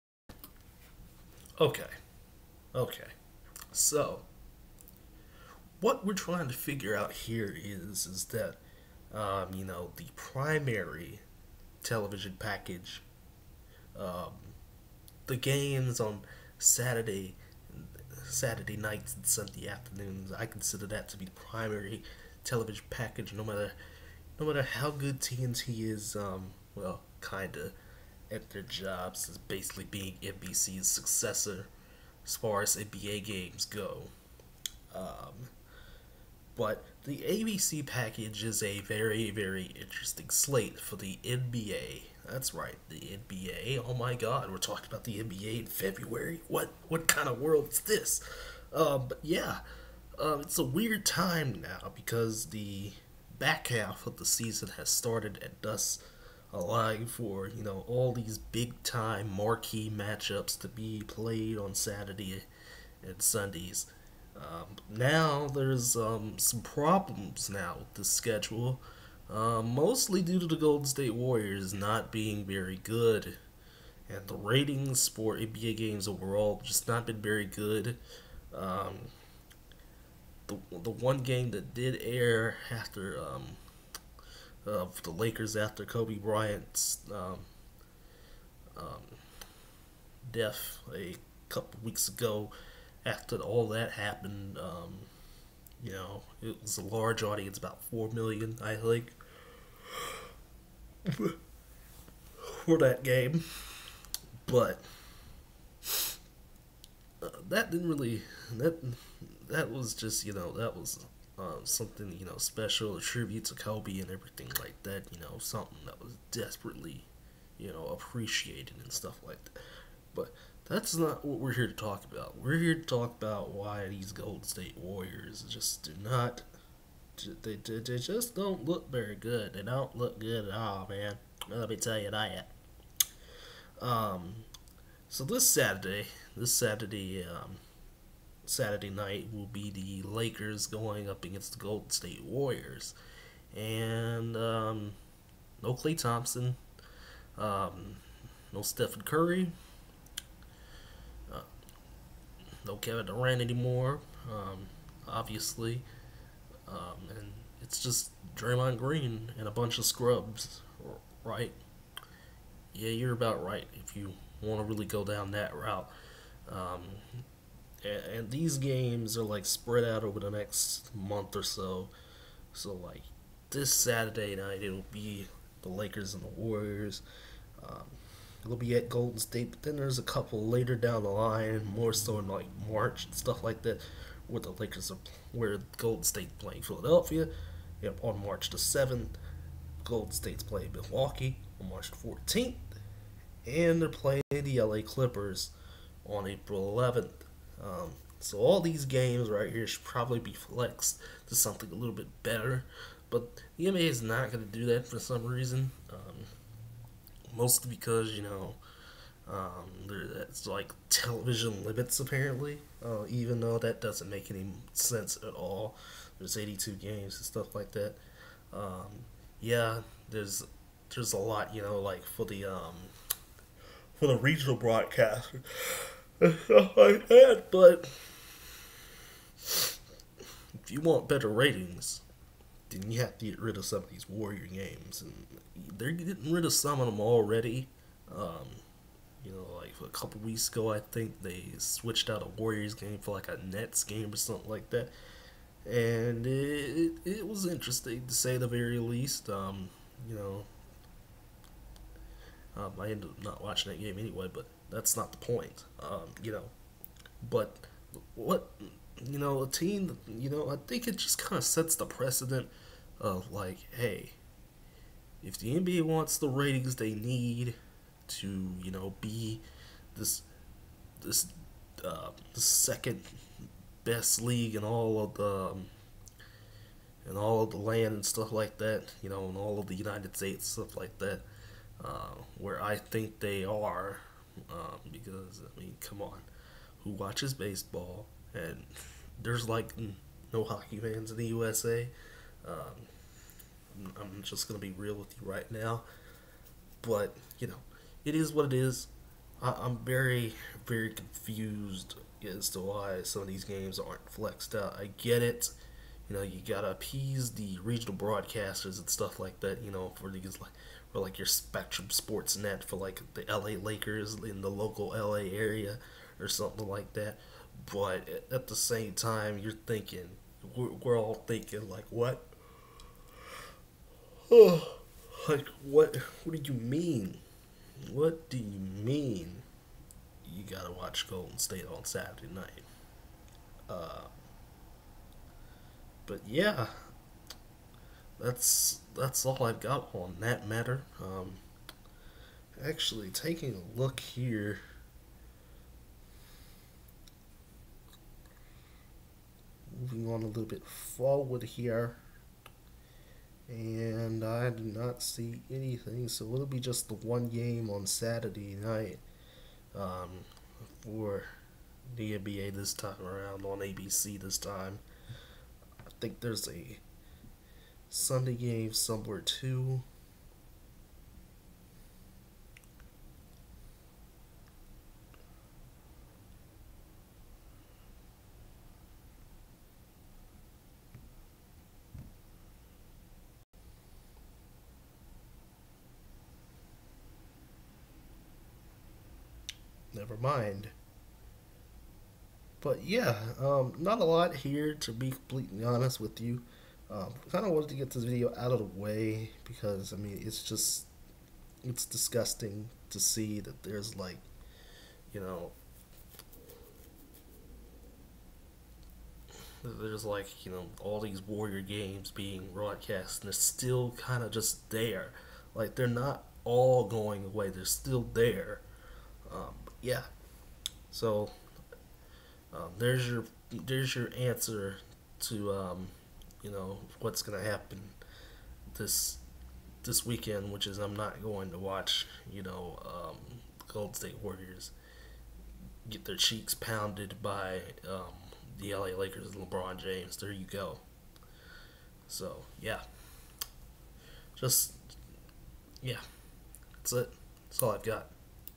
Why The Golden State Warriors Do Not Equal Good Ratings for the NBA
yeah let's not watch the Warriors